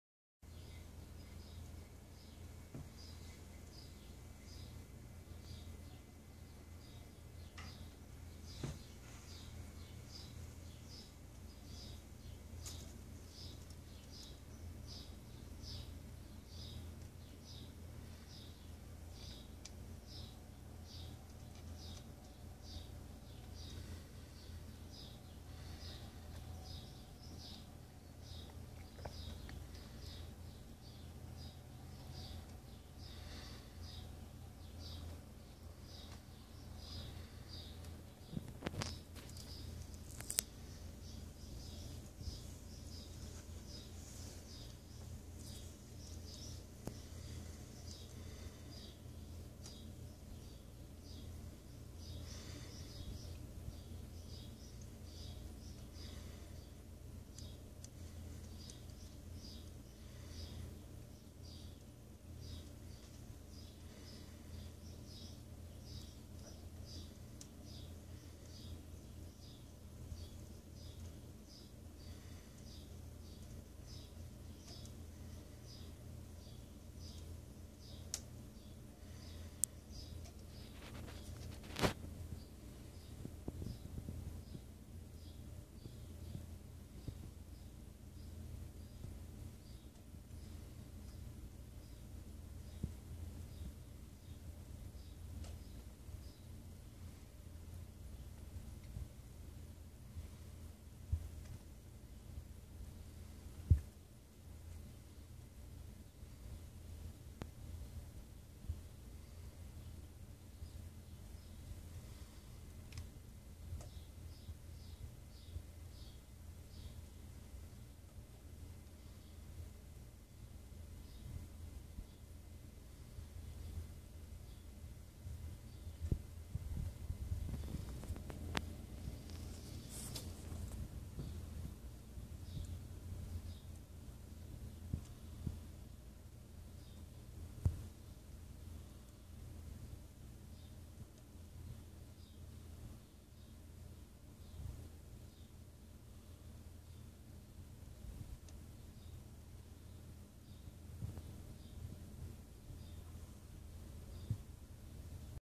Honduran Milk Snake Slithers into Soaking Dish
This is my baby Honduran Milk Snake, Ares, slithering into his water dish for a soak.  As you can see, he is an amazing and beautiful creature!  :)